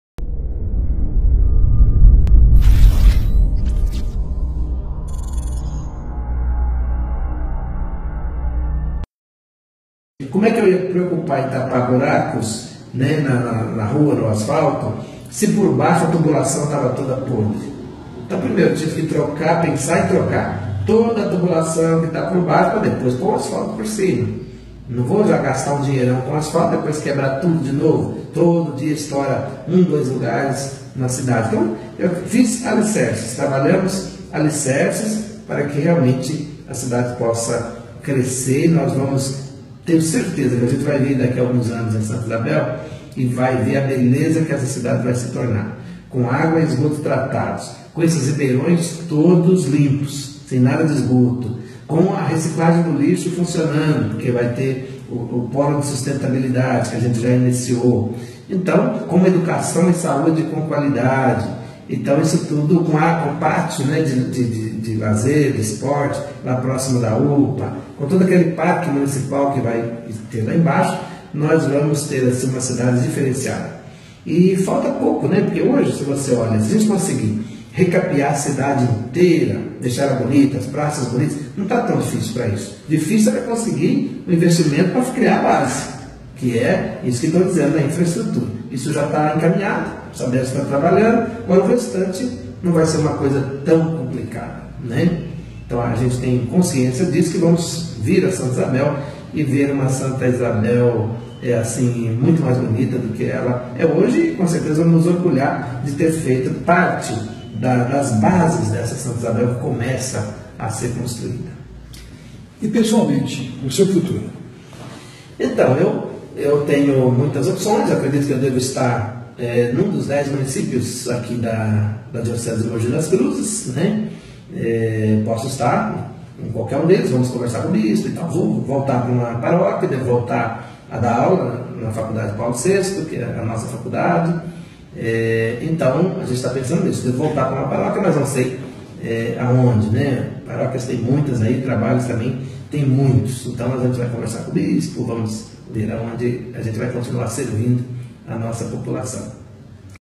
Parte 3 - Prefeito Pe. Gabriel Bina renuncia a pré-candidatura à Prefeitura de Santa Isabel.
Prefeito faz uma análise dos avanços e conquistas alcançadas na atual gestão. Esse vídeo foi dividido em três partes.